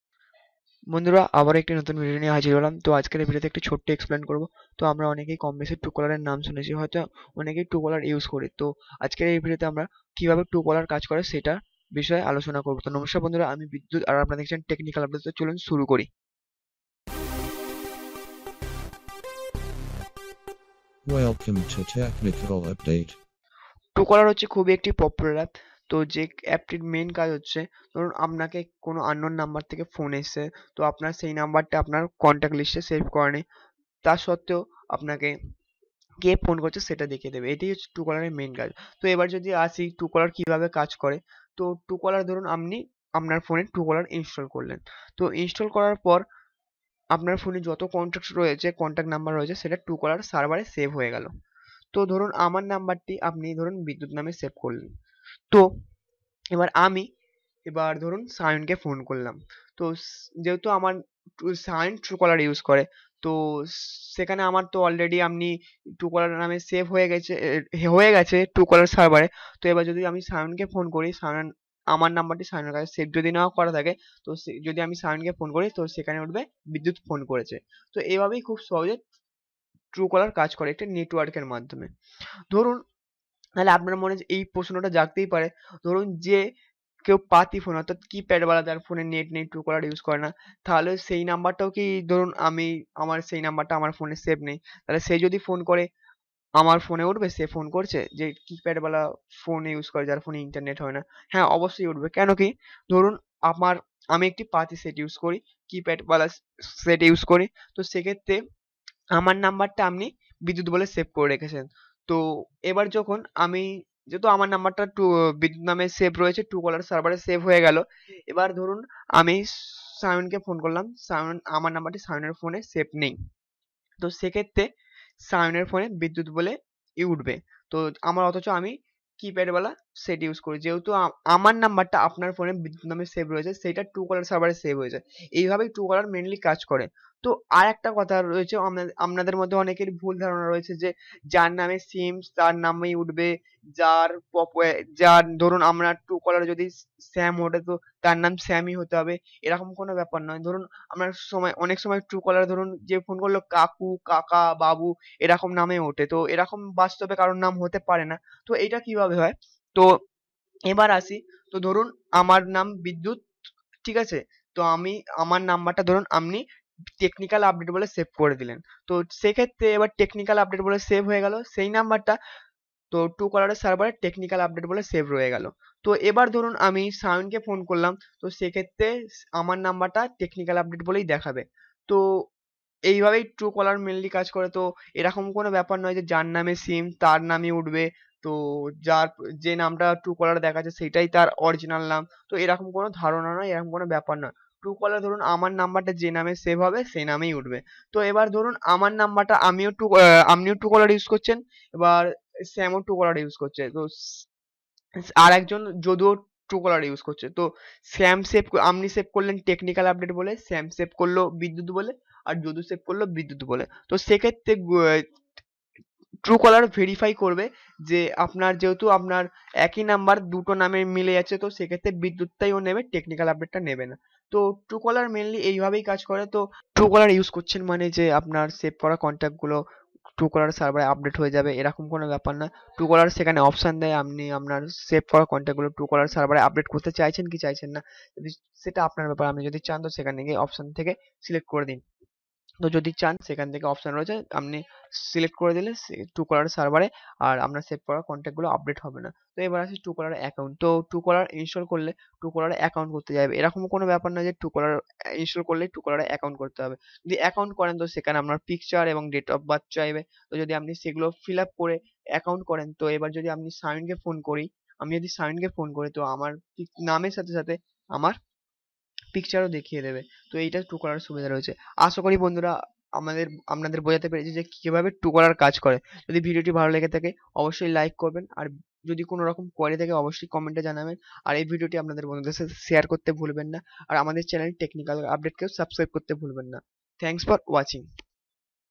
How Truecaller works?[In Bengali]
এই Video এর মাধ্যমে আমি দেখাব How Truecaller works?